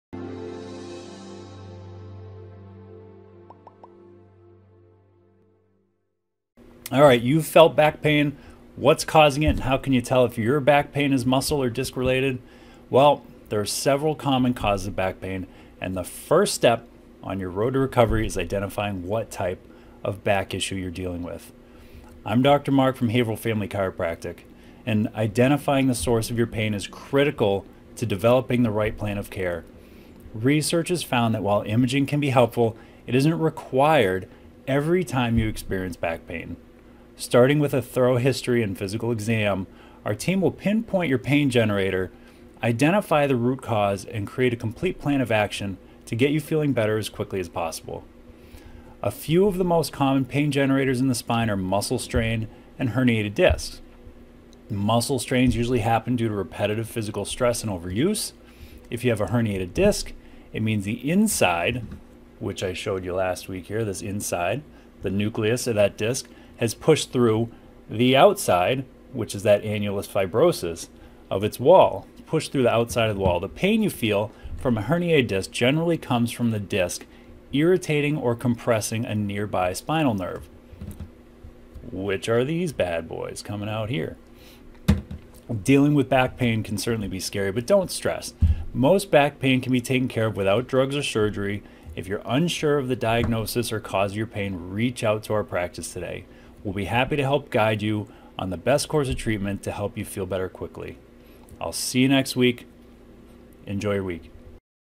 Is It a Muscle or a Disc
Find out the best way to determine whether your pain is coming from a disc, or if it's a muscle problem.